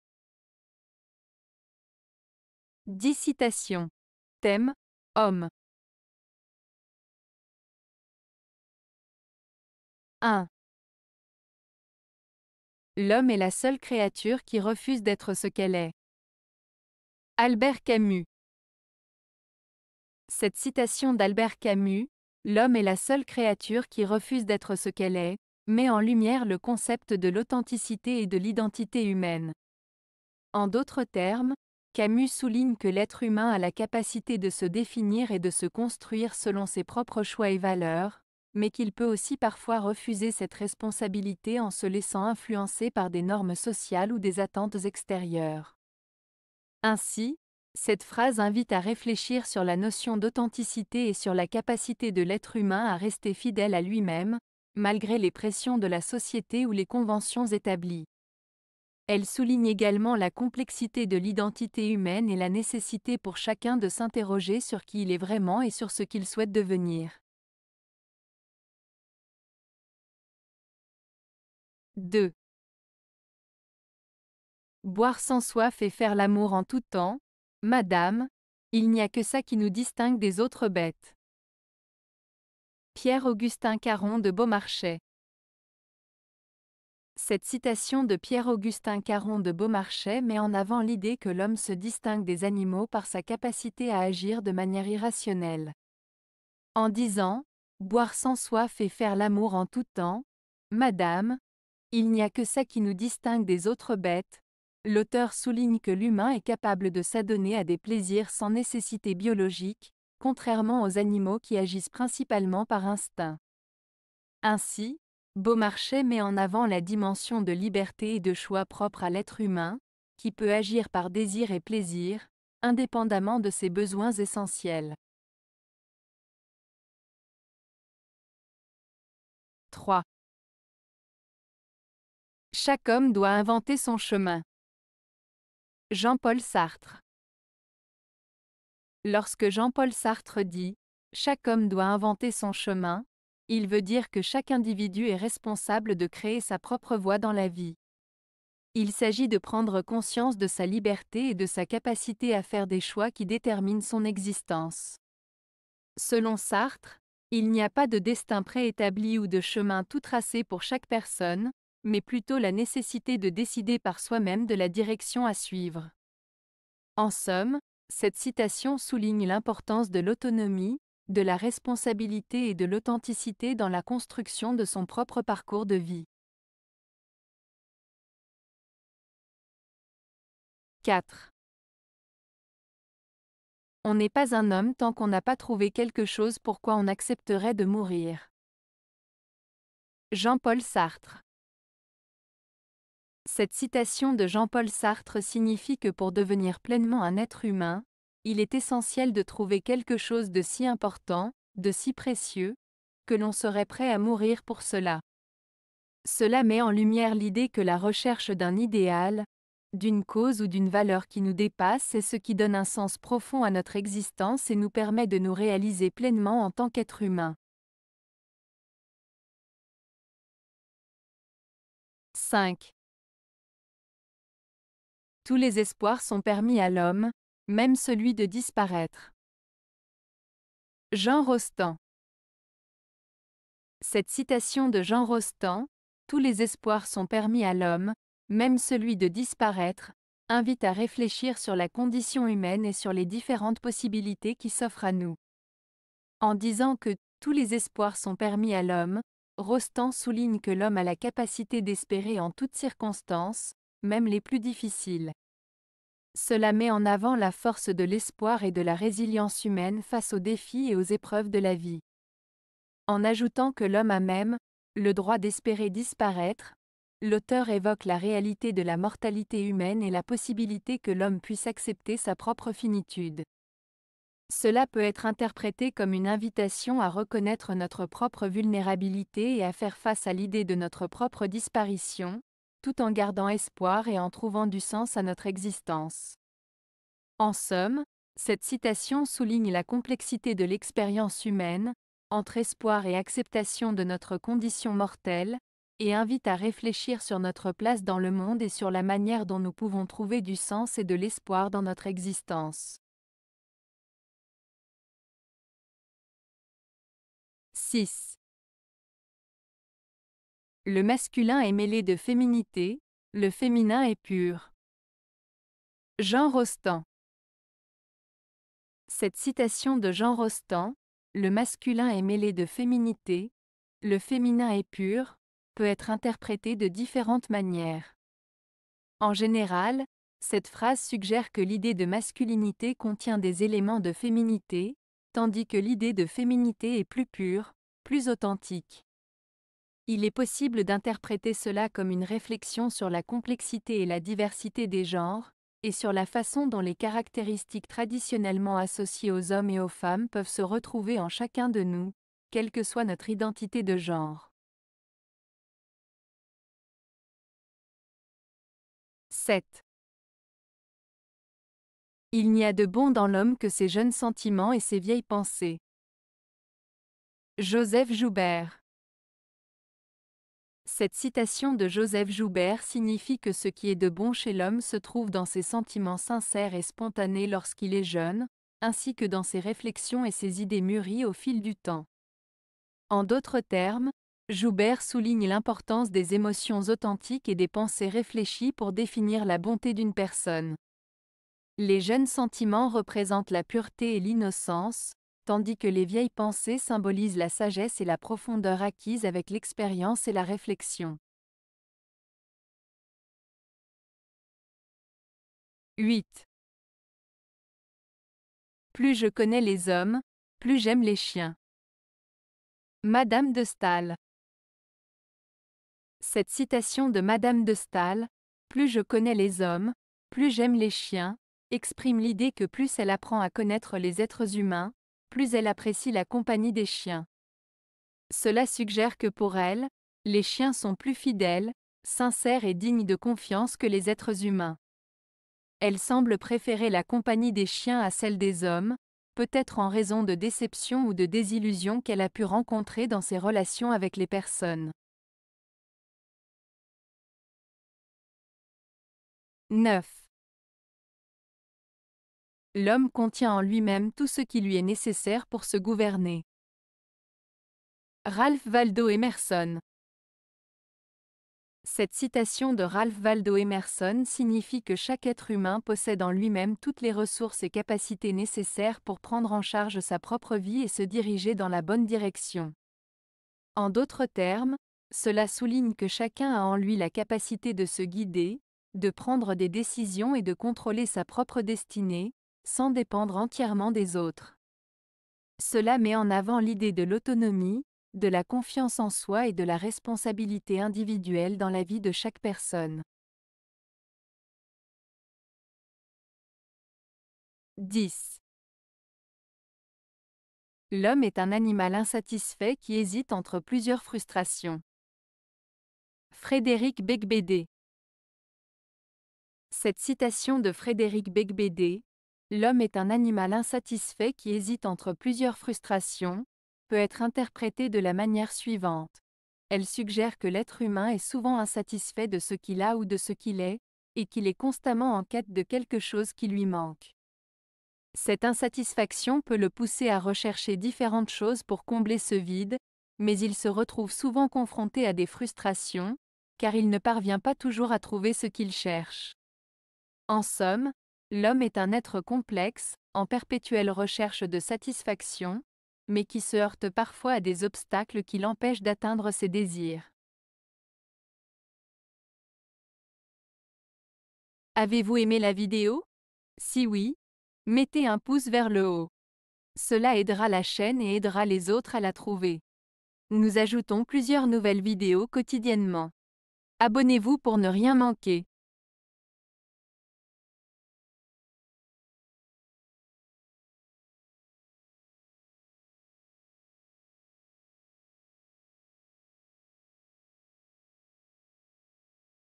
Citations - Homme (8 de 19)
00:00 Introduction
00:07 Albert Camus - L'homme est la seule créature qui refuse d'être ce qu'elle est.
01:09 Pierre-Augustin Caron de Beaumarchais - Boire sans soif et faire l'amour en tout temps, madame, il n'y a que ça qui nous distingue des autres bêtes.
02:07 Jean-Paul Sartre - Chaque homme doit inventer son chemin.
02:58 Jean-Paul Sartre - On n'est pas un homme tant qu'on n'a pas trouvé quelque chose pour quoi on accepterait de mourir.
03:39 Jean Rostand - Tous les espoirs sont permis à l'homme, même celui de disparaître.
05:05 Jean Rostand - Le masculin est mêlé de féminité, le féminin est pur.
05:56 Joseph Joubert - Il n'y a de bon dans l'homme que ses jeunes sentiments et ses vieilles pensées.
06:43 Mme de Staël - Plus je connais les hommes, plus j'aime les chiens.
07:27 Ralph Waldo Emerson - L'homme contient en lui-même tout ce qui lui est nécessaire pour se gouverner.
08:15 Frédéric Beigbeder - L'homme est un animal insatisfait qui hésite entre plusieurs frustrations.